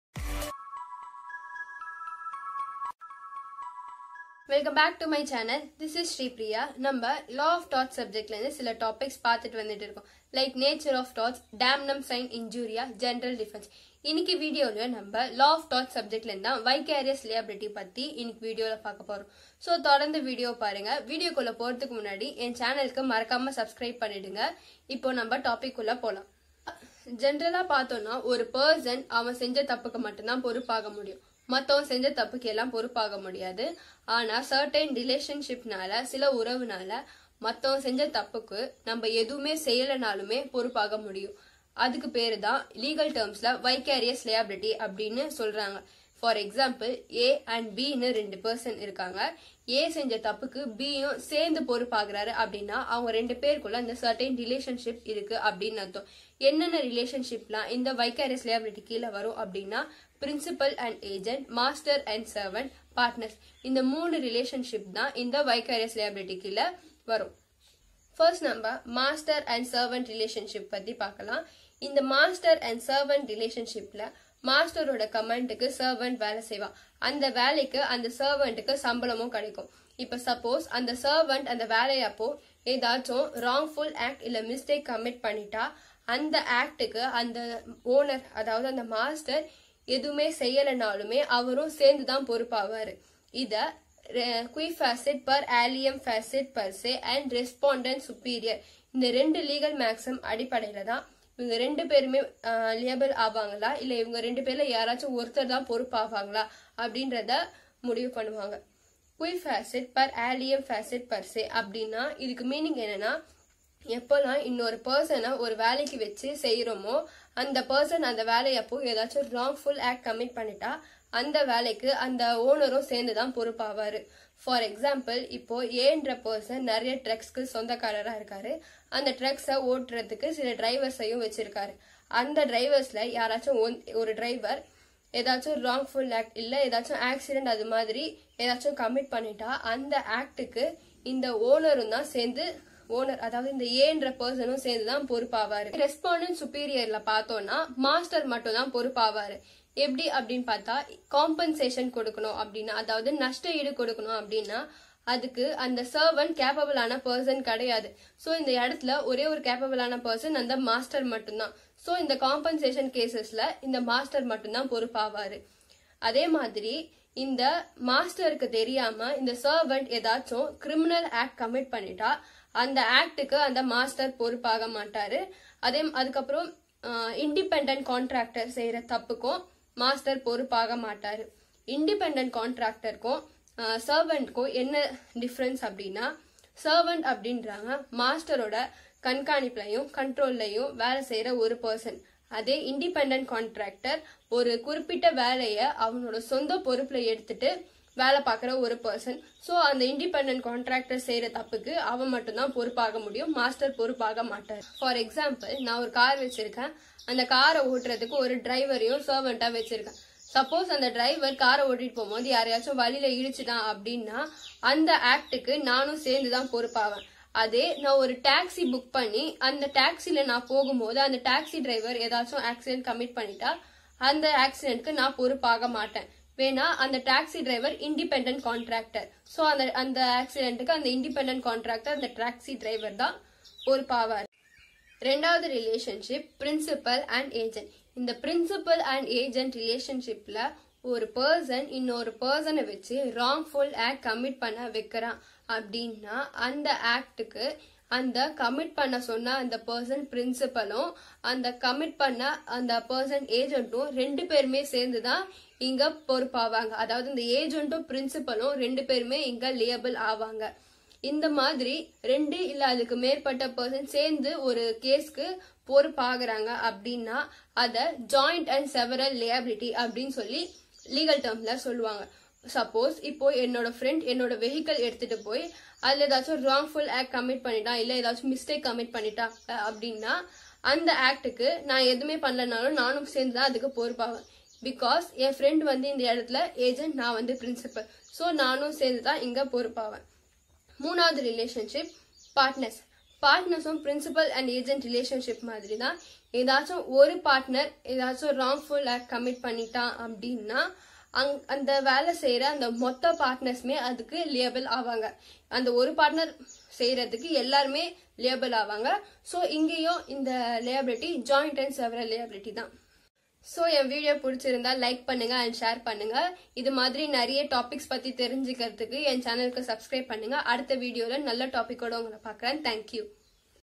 Vicarious liability | Law of Torts | Tamil | Bookish Girl
Hey guys hope you all are doing good. This video is about Vicarious liability from Law of Torts subject,

Do watch the video and if u like my video means give me a like👍.